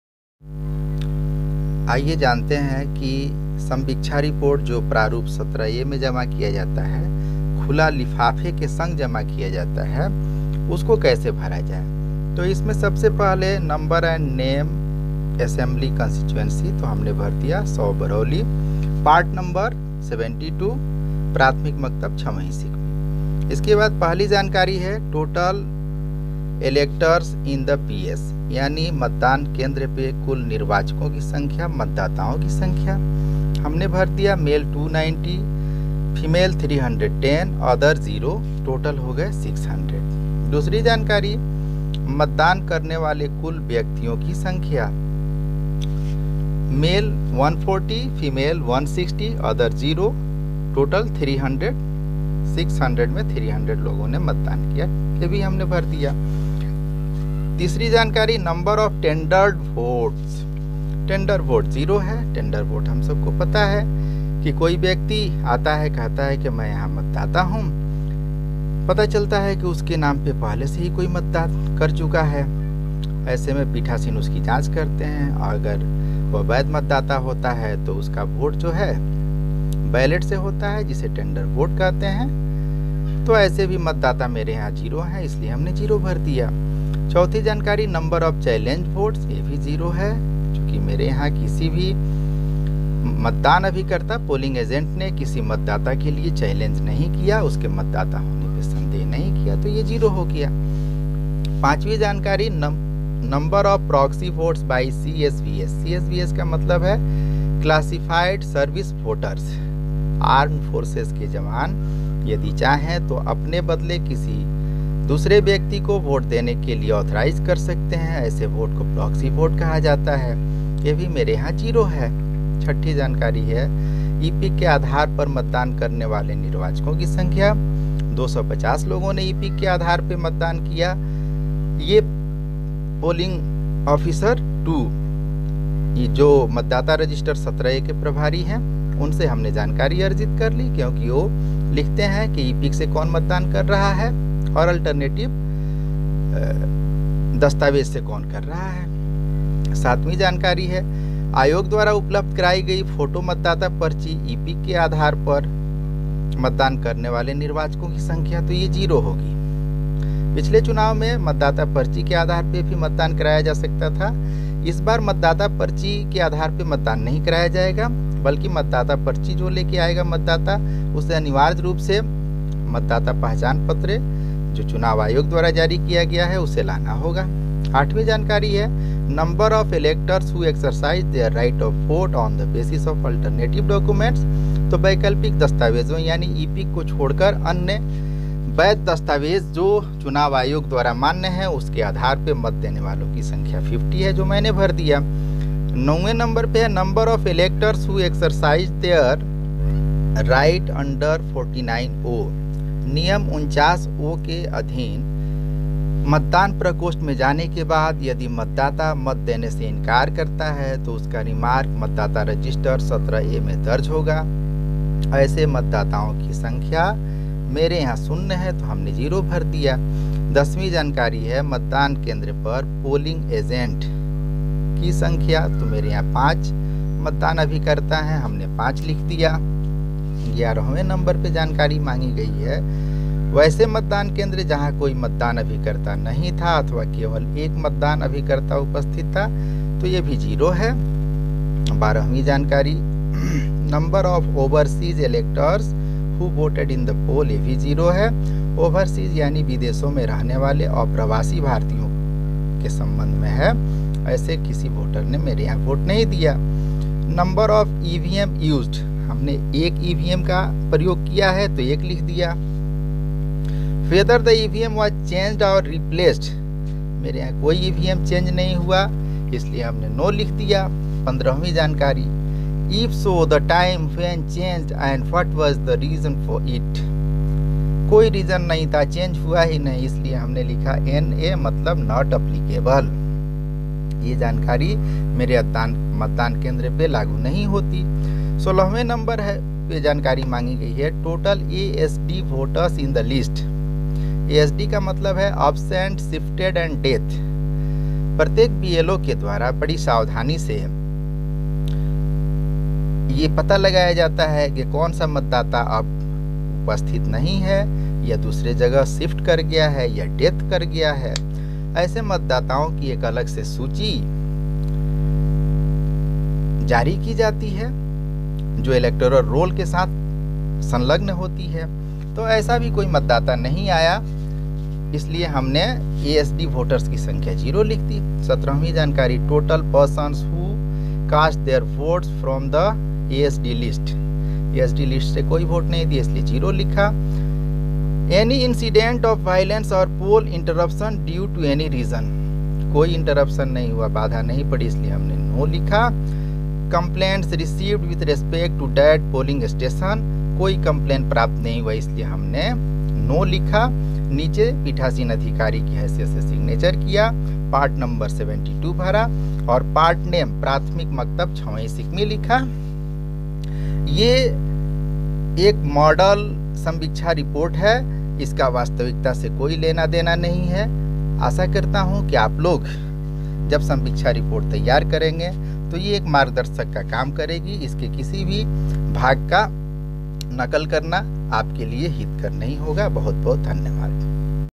scrutiny of form 17A
फार्म 17A एवं अन्य दस्तावेजों की संवीक्षा का भरा हुआ दिखावटी प्रारूप